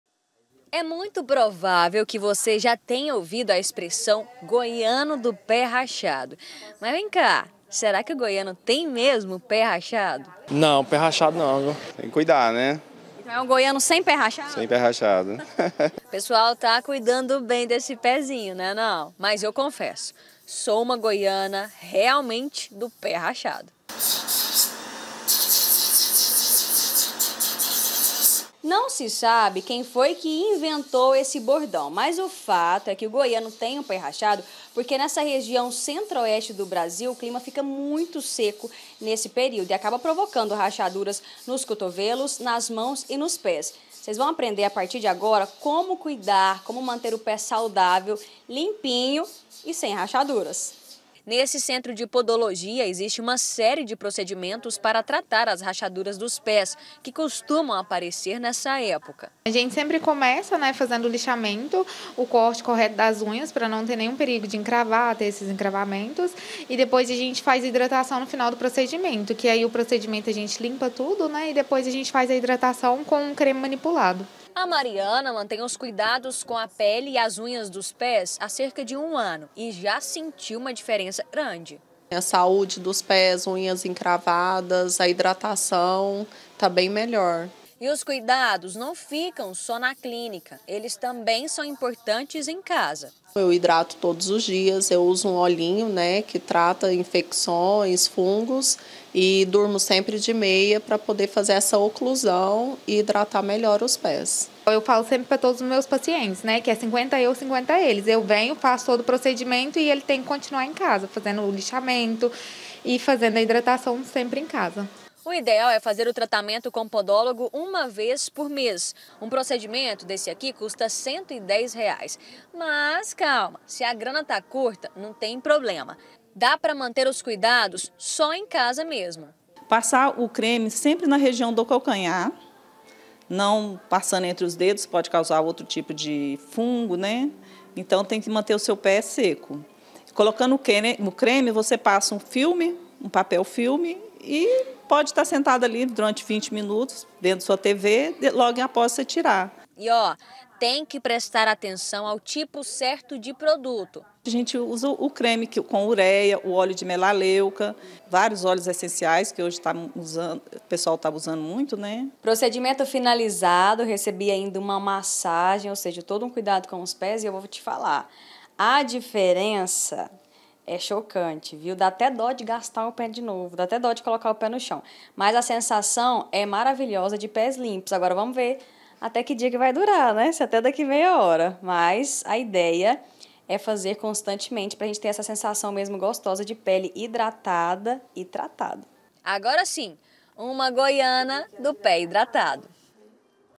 "GOIANO DO PÉ RACHADO": SAIBA O QUE FAZER PARA EVITAR O PROBLEMA
ATIRE A PRIMEIRA PEDRA QUEM NUNA FICOU COM O PÉ RACHADO. PRINCIPALMENTE NESSA ÉPOCA DO ANO, QUANDO A BAIXA UMIDADE DO AR CASTIGA A PELE E OS CABELOS DA GENTE. POR ISSO, A HIDRATAÇÃO É FUNDAMENTAL. ? MAS SERÁ QUE O GOIANO TÁ CUIDANDO DIREITINHO DOS PÉS? A KAMYLLA RODRIGUES FOI ATRÁS DA RESPOSTA.